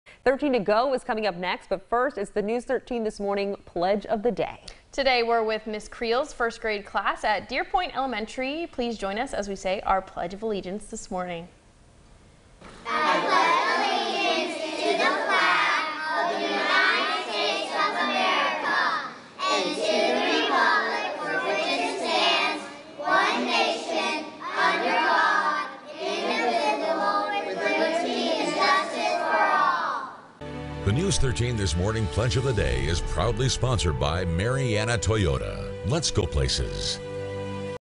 The Daily Pledge: Ms. Creel's 1st Grade Class
The daily pledge of allegiance was recited by Ms. Creel's 1st Grade Class at Deer Point Elementary School!